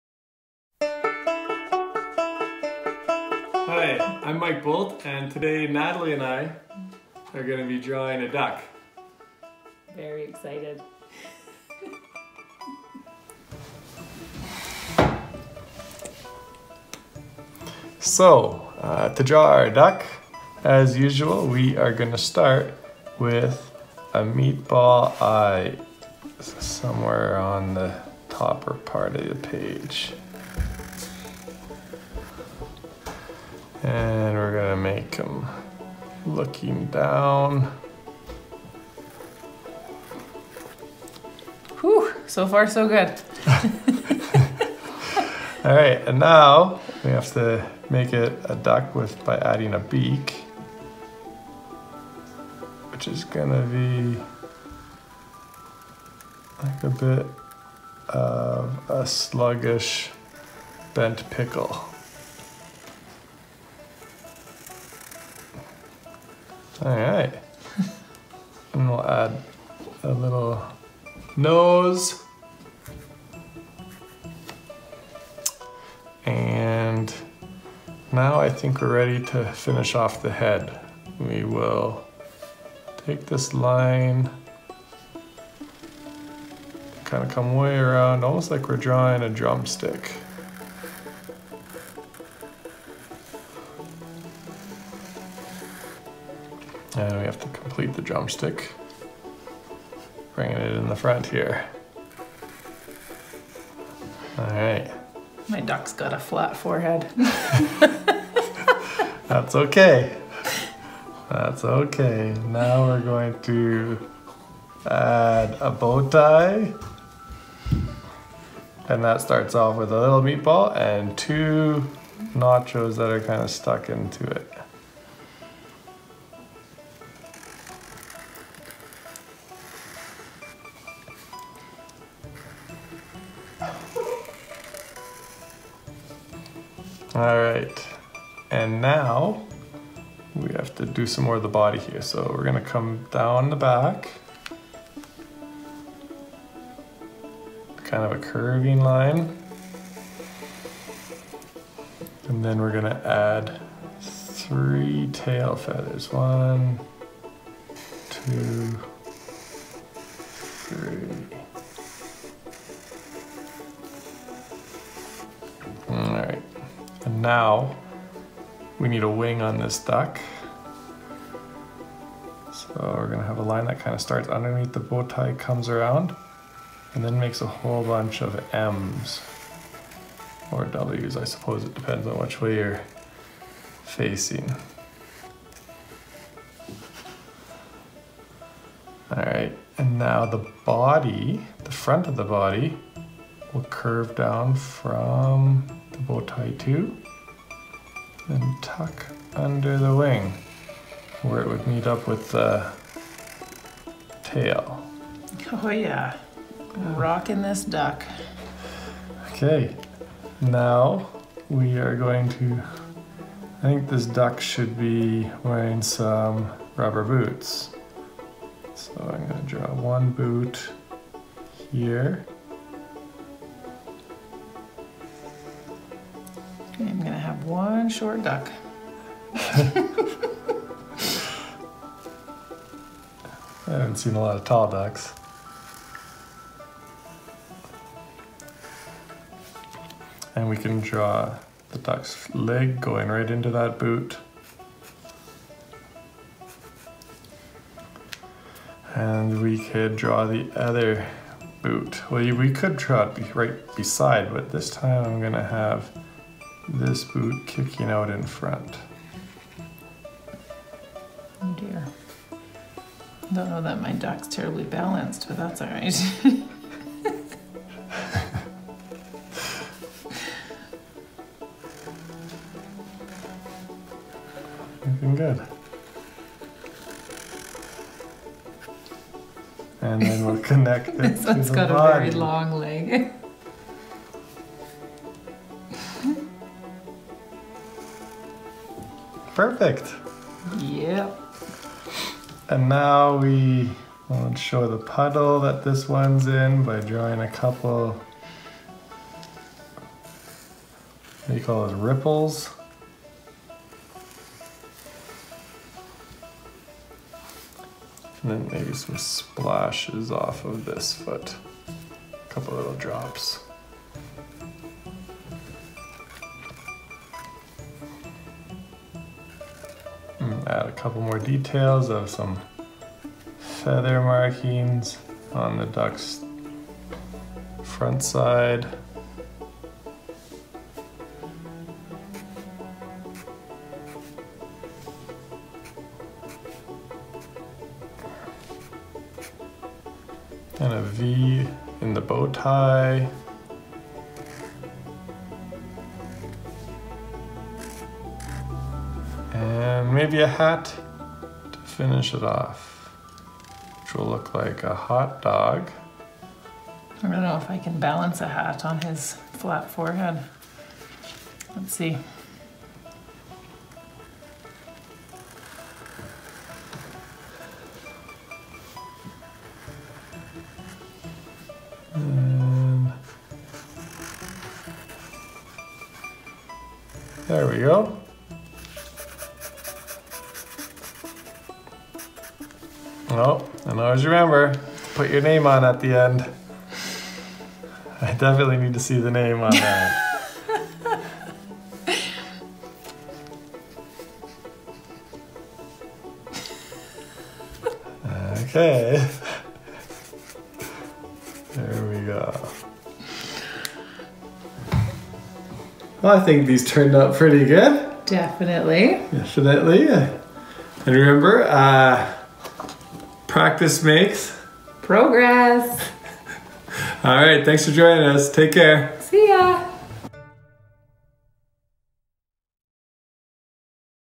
Doodle Time with Mike Boldt ep.4 A DUCK
Hi! I'm Author Illustrator Mike Boldt and today I'm joined by the one who initiated this whole doodling video thing, my beautiful wife. Join us as we doodle up a couple of quacky ducks!

For more drawing activities, check out the other episodes on my channel.